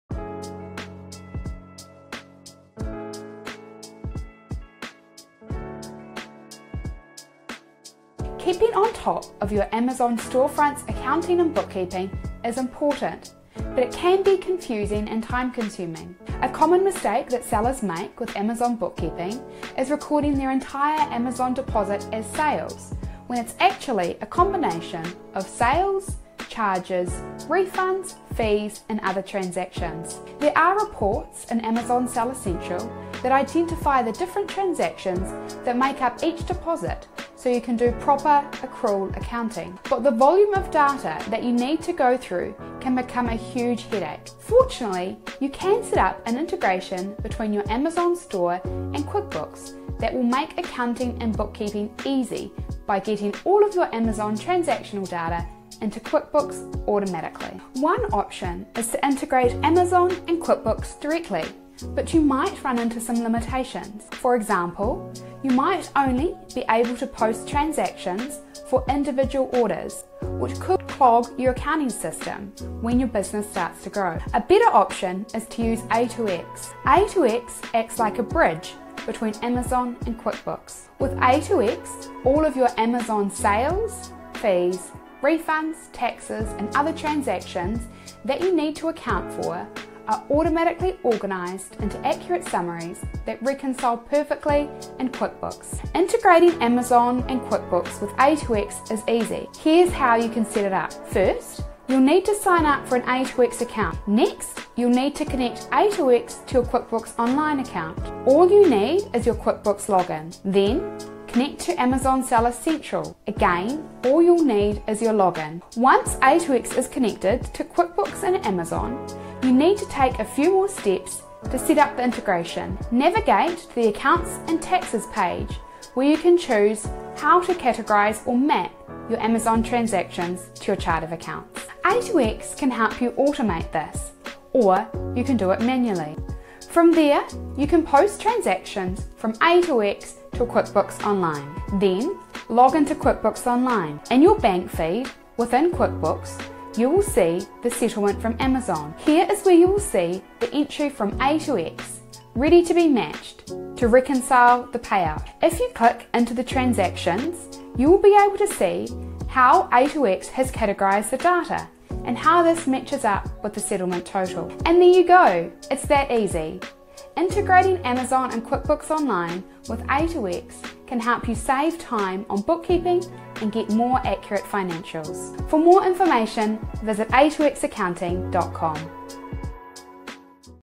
How to integrate Amazon Seller Central and QuickBooks Online for accurate accounting
Looking for a way to integrate Amazon Seller Central with QuickBooks Online? We’ll show you how!

Watch to learn:
- How integrating Amazon Seller Central with QuickBooks Online using A2X can help make your Amazon bookkeeping quick, easy, and accurate
- Why integrating Amazon and QBO directly might not be a good idea for your store (hint: it could clog your accounting system)
- How to use A2X to integrate Amazon Seller Central with QuickBooks Online

Questions?